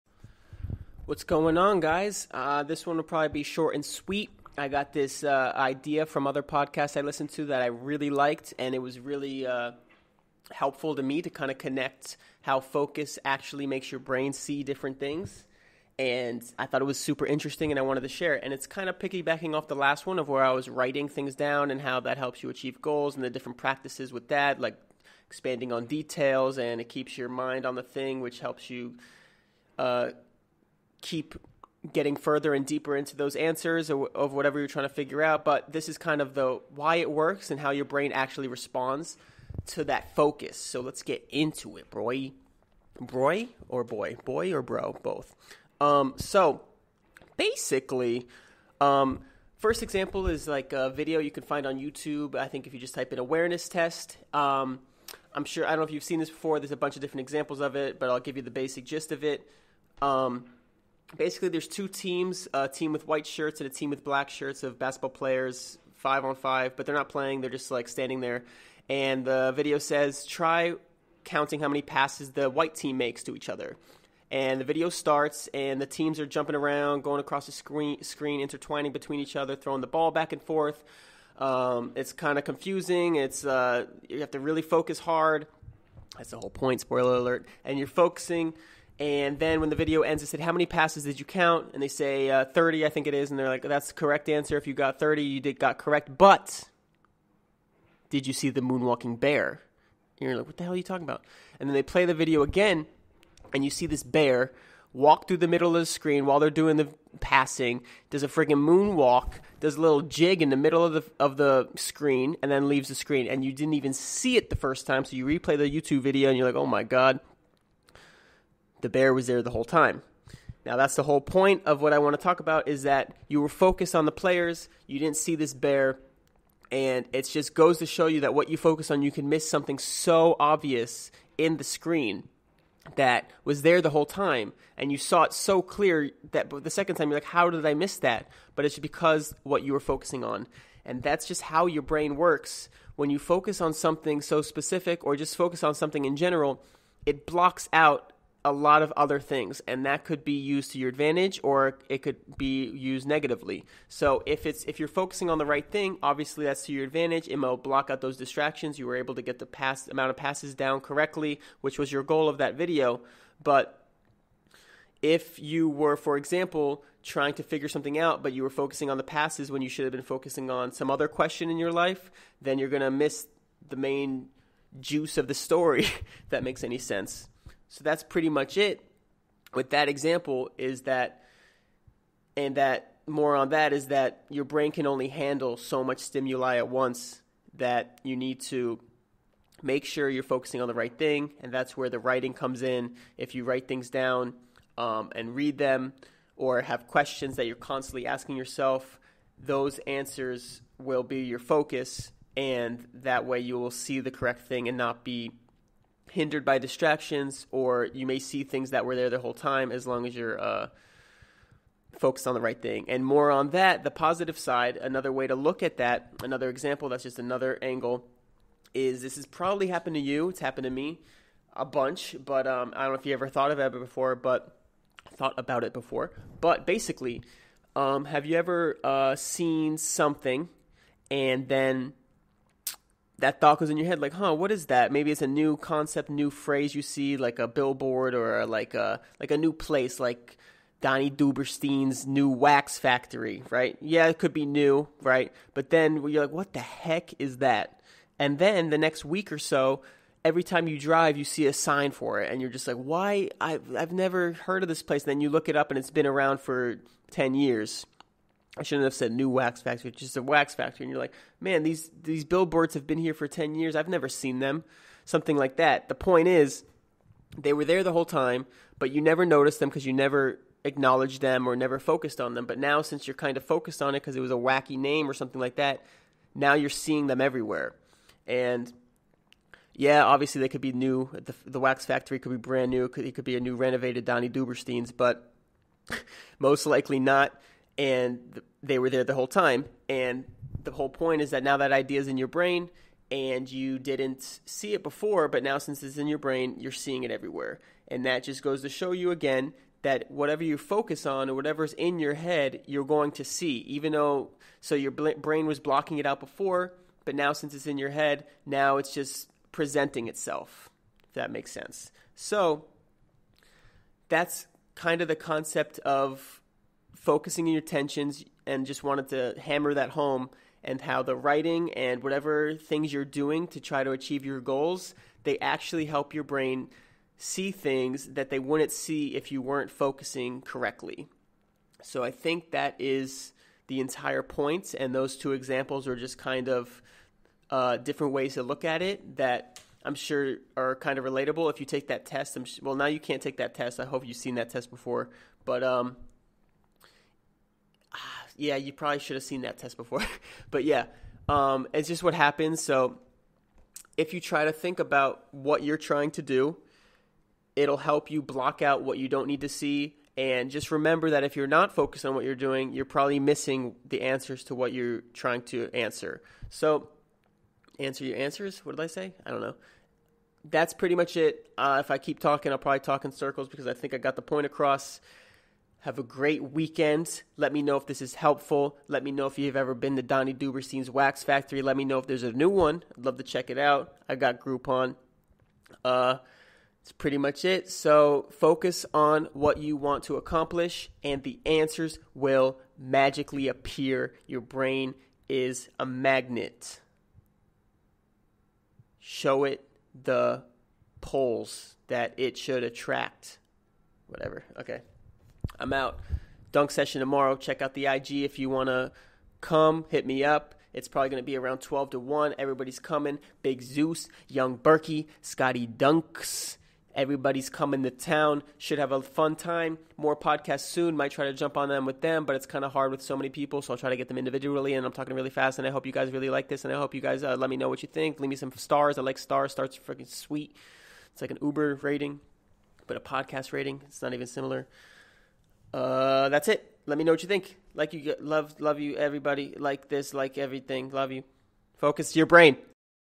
Podcast #17 - Focus intentions correctly, your brain blocks the rest out
Make sure you're focusing on what you want to accomplish. Your brain can only handle so much and will block out the rest. Use it towards your advantage and answers will present themselves.